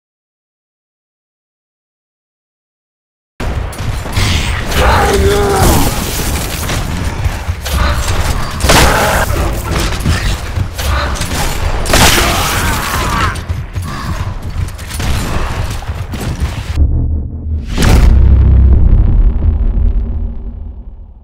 ONSCREEN TRIP BLINDY! o.O
I have no idea how I hit this since I never even pick up the sniper normally lmao!

Made GCOTD for the first time and probably the last GG.